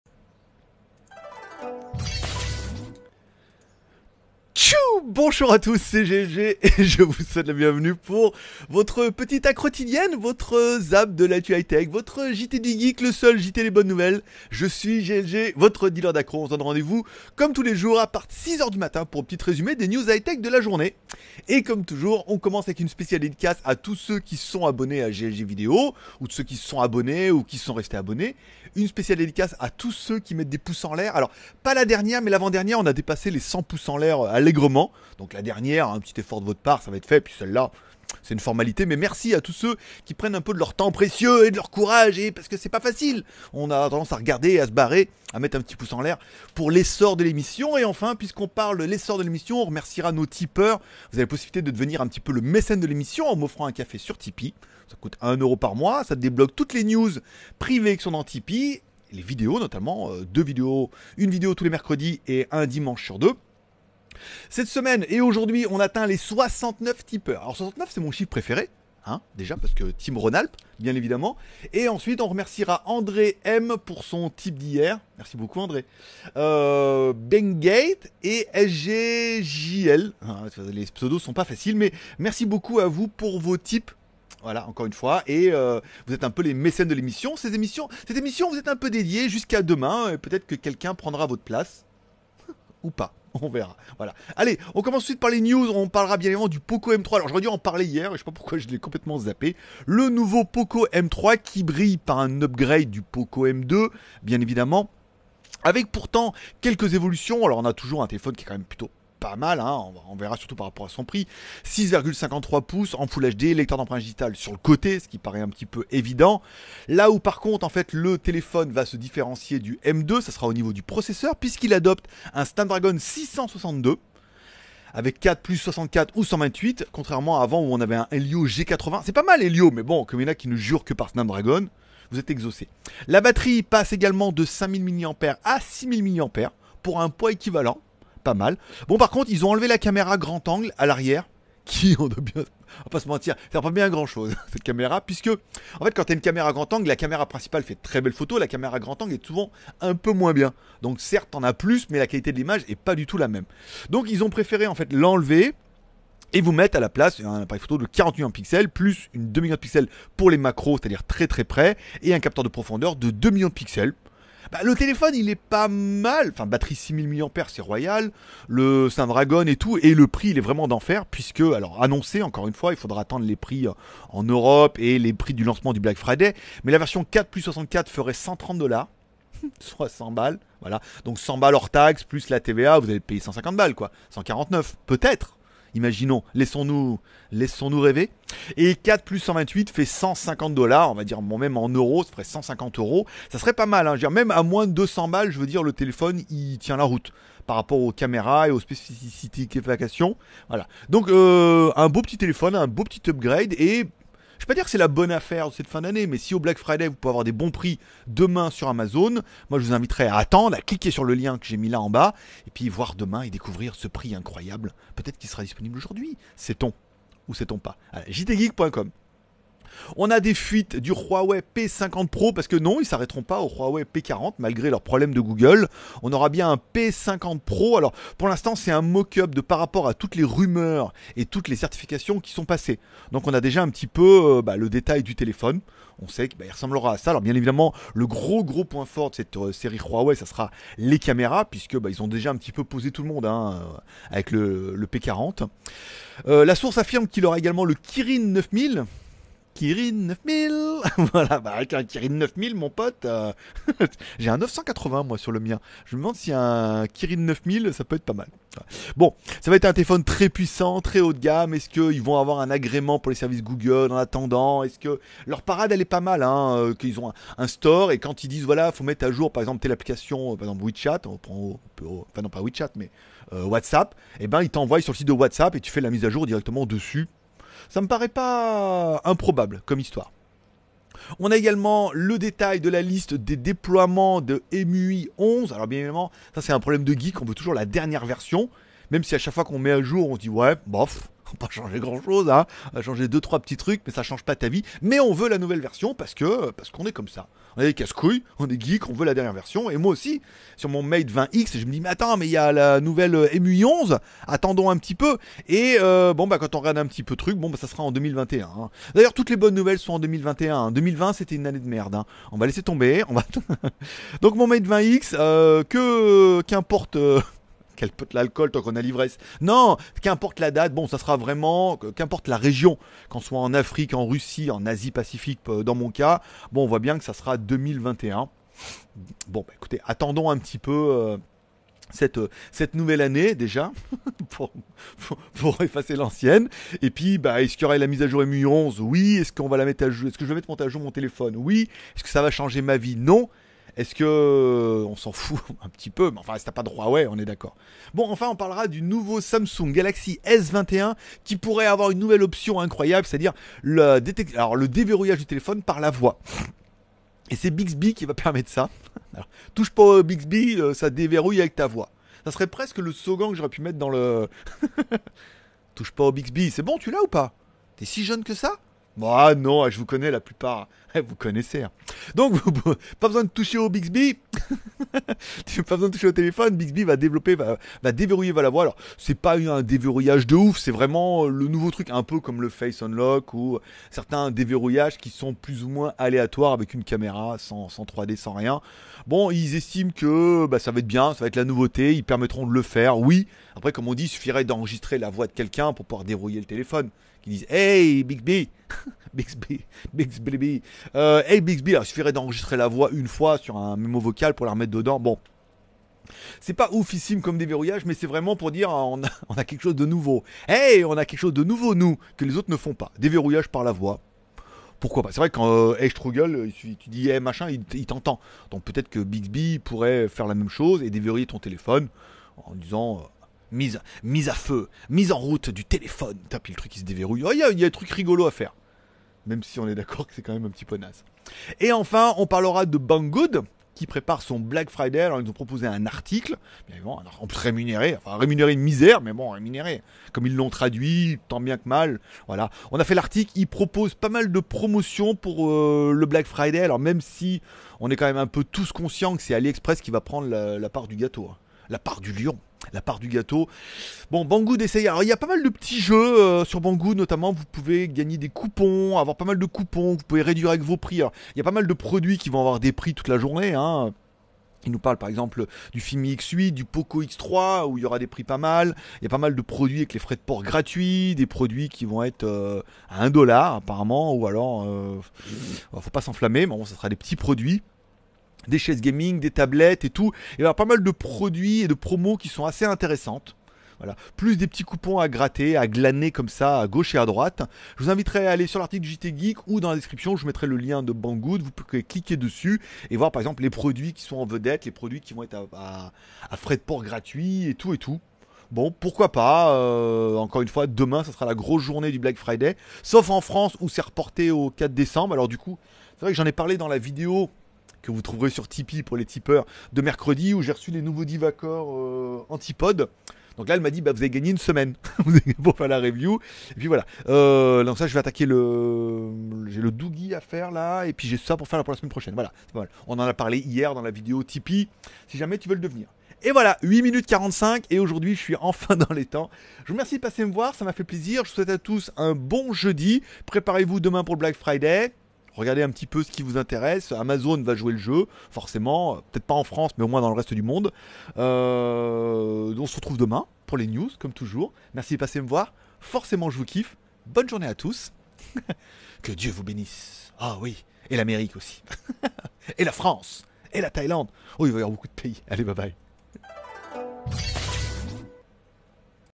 Jt du Geek 26 Nov By GLG
Le Zap actu High-Tech by GLG , un petit résumé en Vidéos des dernières news tech et smartphone du Jtgeek @GLGreviews

By GLG